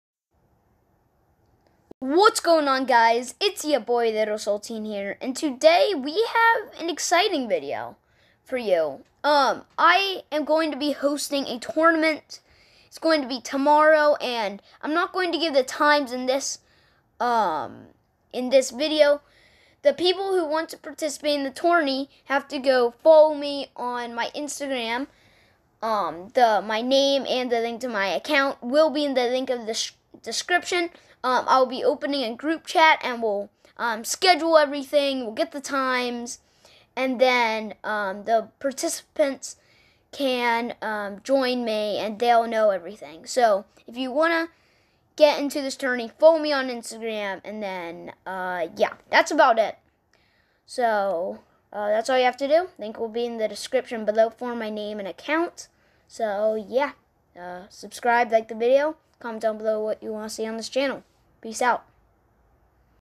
I'm Hosting a Grand Battle Royale Tournament (READ DESCRIPTION)
Steps to get in my tournament:

2. DM that you want to participate
3. I will make a group chat telling the participants the times and rules of the tourney.